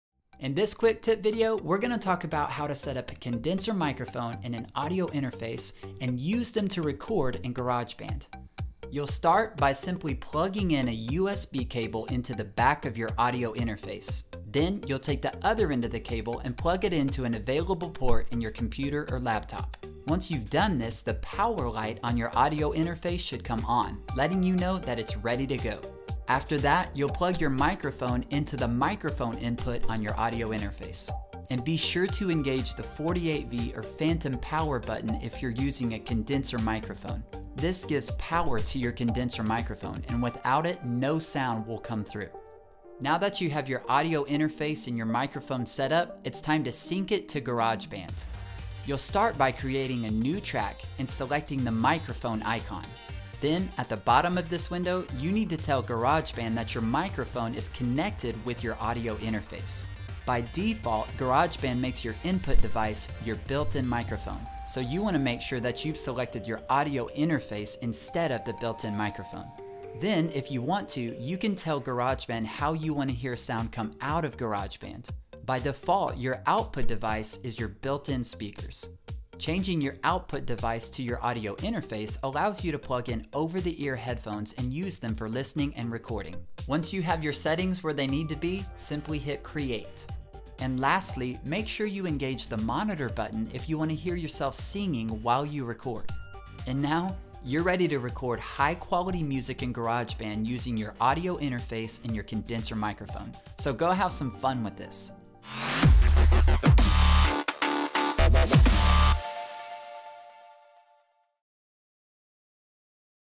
How To Connect Your Recording Gear To GarageBand
Learn how to plug in and use your microphone and audio interface within GarageBand

Get my free 5 part quick start guide to GarageBand

For weekly songwriting & music production training become a Songwriting Studio email subscriber at:
TheSongwritingStudio.com